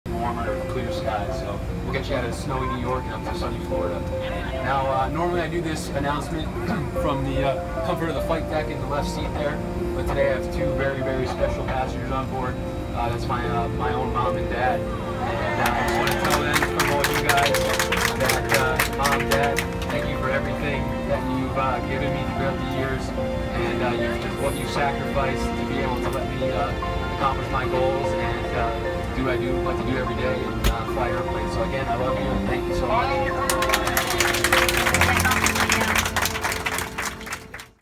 Pilot has onboard surprise for his parents | Humankind
Peter Cappio surprises his parents on a flight from New York to Florida by thanking them on the PA system for supporting his dream of being an airline pilot.